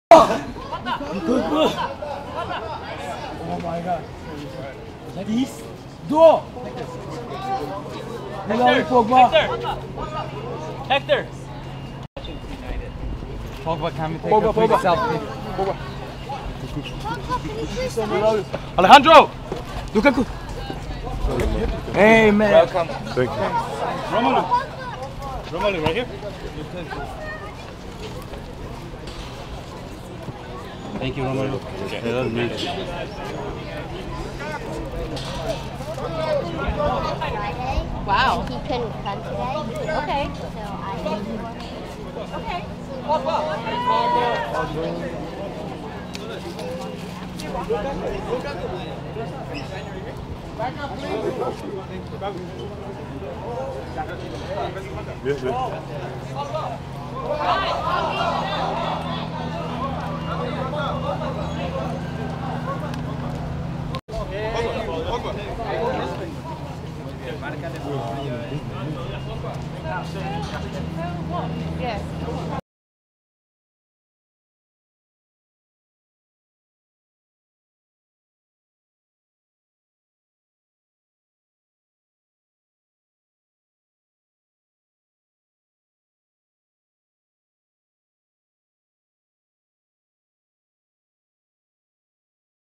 Romelu Lukaku Arrives At His First Manchester United Training Session - Manchester United Tour 2017
Footage of Romelu Lukaku arriving for his first training session as a Manchester United player in Los Angeles. The club's new recruit arrived for the training session at UCLA with Paul Pogba.

Thanks